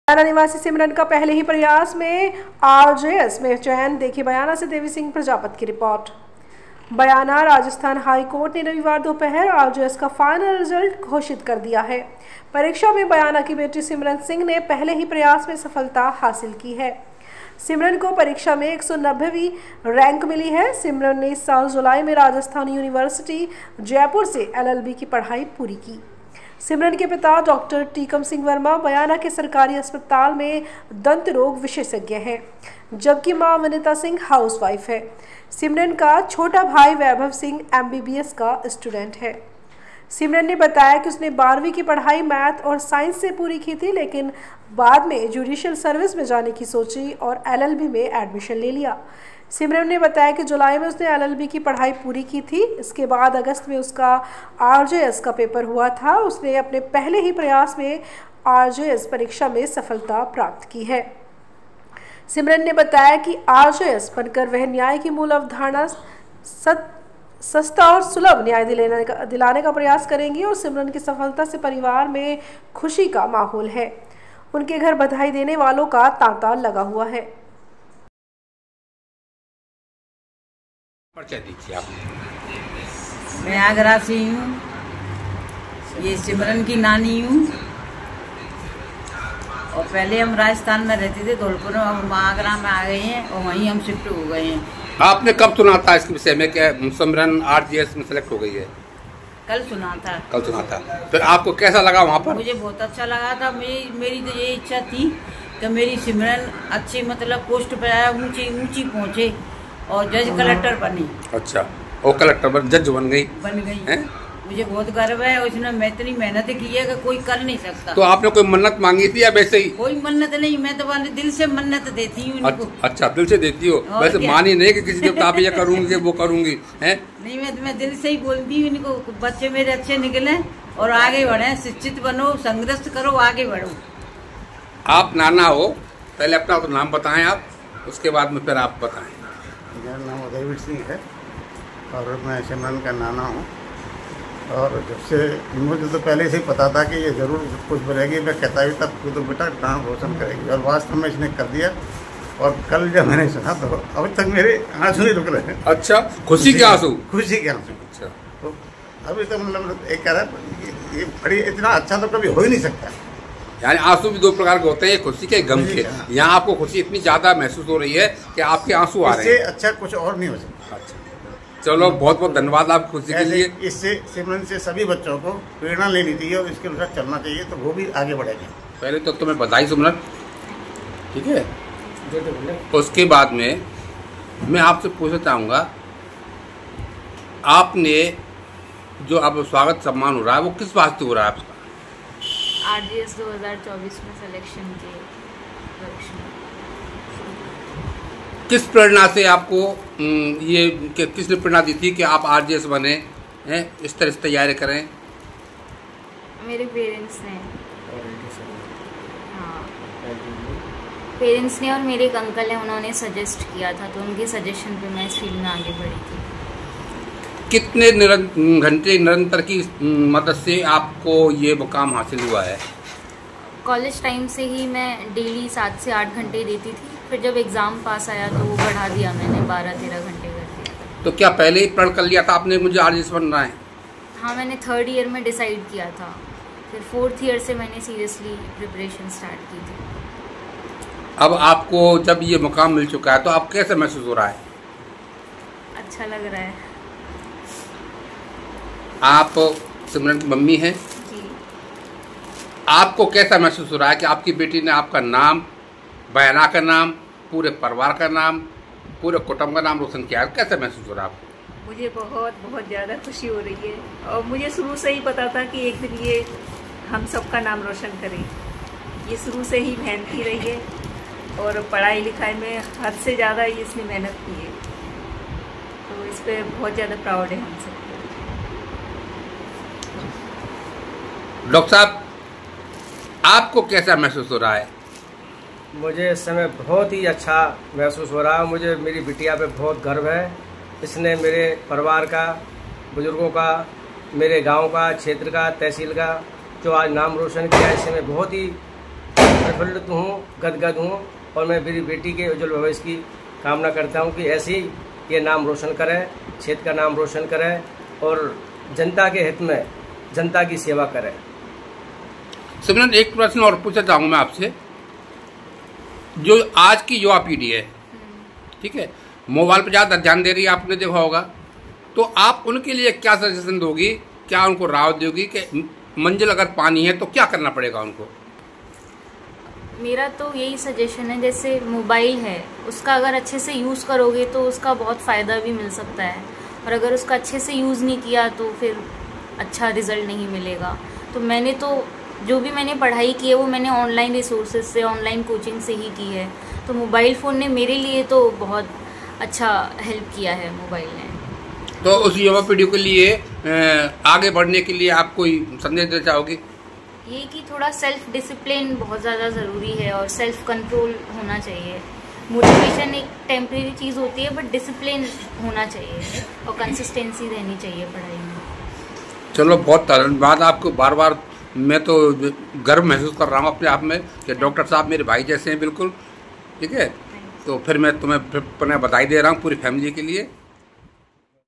बयाना# निवासी सिमरन का पहले ही प्रयास में आरजेएस में चयन#gnewsindia
बयाना निवासी सिमरन का पहले ही प्रयास में आरजेएस में चयन ।

बयाना - राजस्थान हाई कोर्ट ने रविवार दोपहर आरजेएस का फाइनल रिजल्ट घोषित कर दिया।
परीक्षा में बयाना की बेटी सिमरन  सिंह ने अपने पहले ही प्रयास में सफलता हासिल की है। सिमरन को परीक्षा में 190वीं रैंक मिली है। सिमरन ने इसी साल जुलाई में राजस्थान यूनिवर्सिटी जयपुर से एलएलबी की पढ़ाई पूरी की है। सिमरन के पिता डॉ. टीकम सिंह वर्मा बयाना के सरकारी अस्पतालमें दंत रोग विशेषज्ञ हैं। जबकि मां विनीता सिंह हाउसवाइफ हैं। सिमरन का छोटा भाई वैभव सिंह एमबीबीएस स्टूडेंट है।
सिमरन ने बताया कि उसने 12वीं की पढ़ाई मैथ साइंस से पूरी की थी। लेकिन बाद में उसने ज्यूडिशल सर्विस में जाने की सोची और एलएलबी में एडमिशन ले लिया। सिमरन ने बताया कि जुलाई में उसने एलएलबी की पढ़ाई पूरी की थी। इसके बाद अगस्त में उसका आरजेएस का पेपर हुआ था। उसने अपने पहले ही प्रयास में आरजेएस परीक्षा में सफलता हासिल की है। सिमरन ने बताया कि आरजेएस बनकर वह न्याय की मूल अवधारणा सस्ता और सुलभ न्याय दिलाने का प्रयास करेगी। सिमरन की सफलता से परिवार में खुशी का माहौल है उनके घर बधाई देने वालों का तांता लगा हुआ है ।